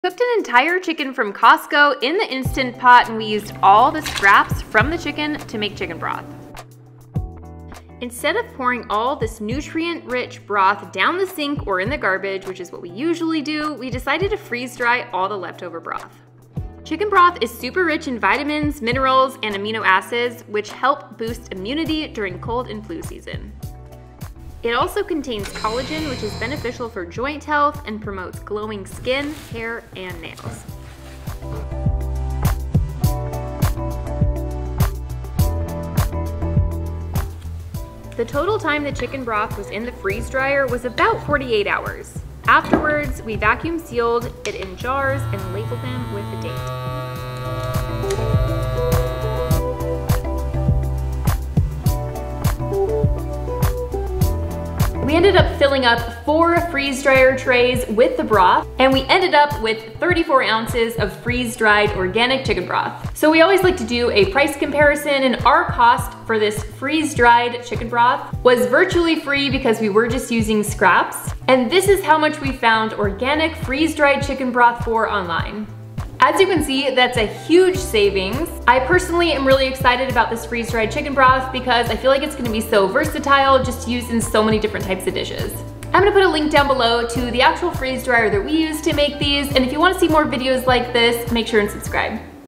Freeze Drying LEFTOVER Chicken Broth | I Saved $300
Freeze-drying chicken broth offers a unique set of benefits, preserving both the nutritional integrity and convenience of this nourishing liquid. Through the freeze-drying process, chicken broth can be transformed into a lightweight, shelf-stable powder while retaining essential nutrients such as collagen, vitamins, and minerals. This preservation method not only extends the broth's shelf life but also makes it a convenient option for backpacking, camping, or emergencies, as it requires minimal storage space and eliminates the need for refrigeration. The resulting powder can be easily reconstituted with water, providing a quick and portable source of hydration, essential amino acids, and immune-boosting compounds. The freeze-drying technique thus combines the nutritional benefits of chicken broth with practicality, offering a versatile and long-lasting option for those seeking a convenient and nutritious addition to their diet.

Stuff we use:
➡️ Vacuum Sealer
➡️ Glass Jars
➡️ Tray Stackers
➡️ Quail Cutting Scissors